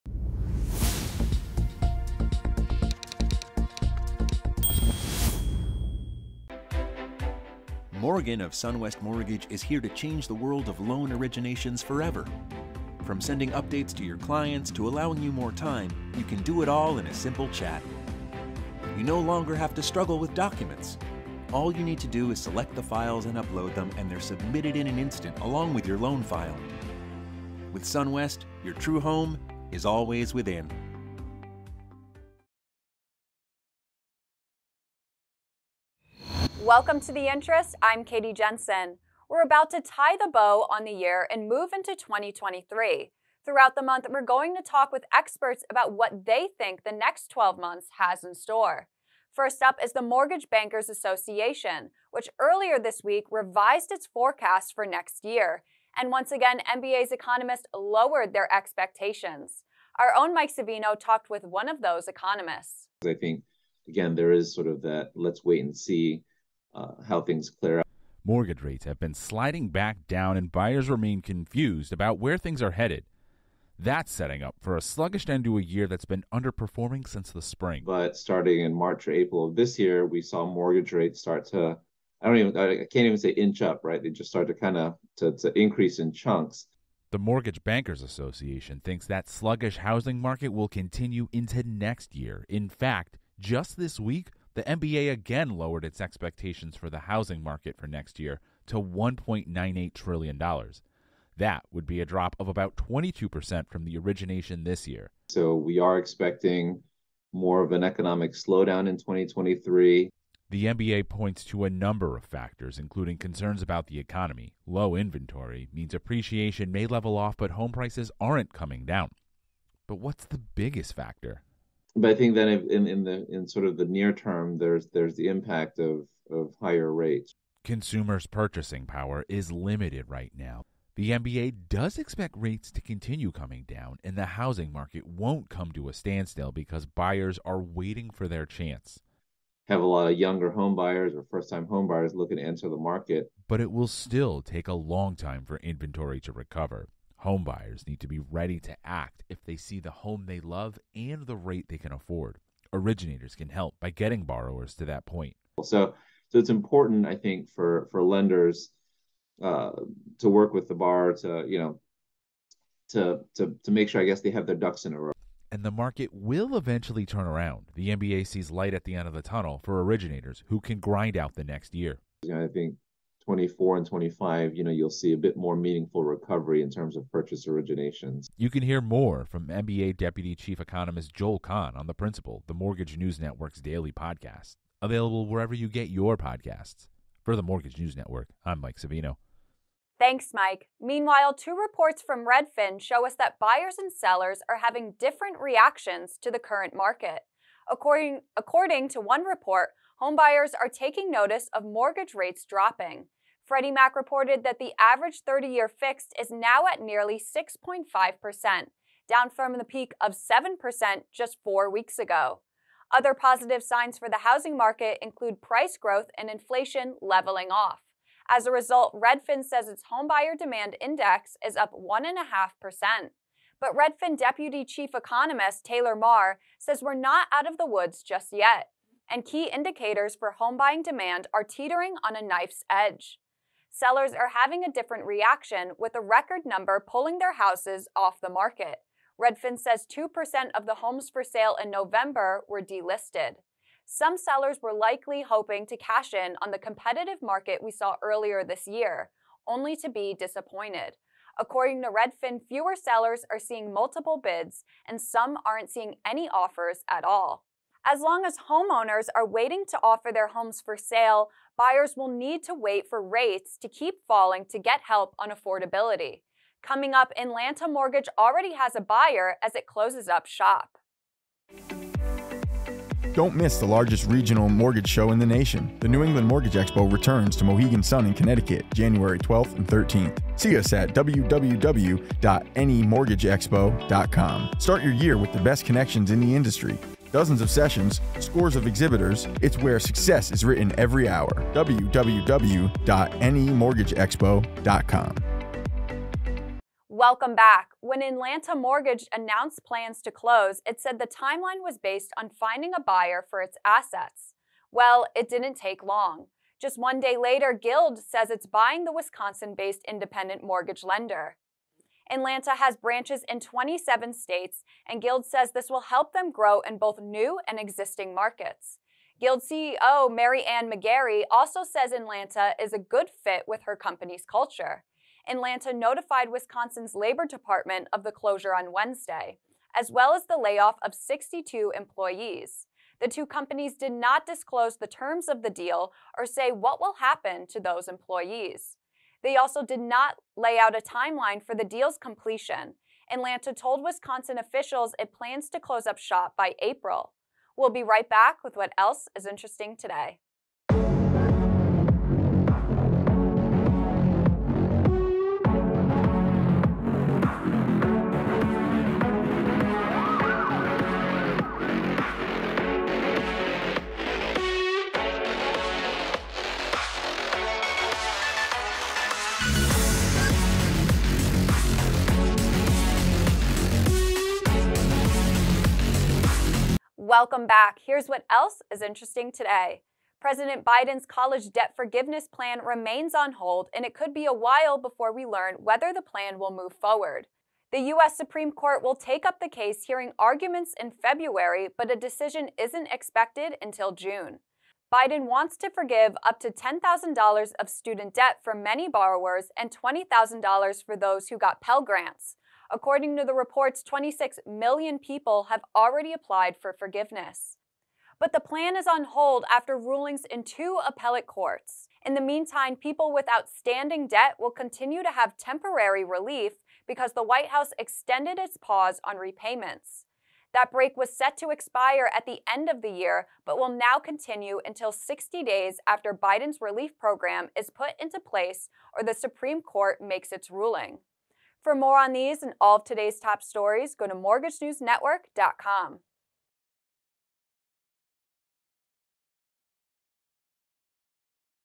MBA Tells Us What To Expect In 2023 | The Interest
to learn about Morgan™: People-powered empathetic technology capable of high level financial transactions saving time and supporting mortgage team members.

On today's TheInterest...
Throughout December, we’ll ask industry experts what they think 2023 will bring. We start things off with the MBA. Meanwhile, buyers and sellers are moving in different directions.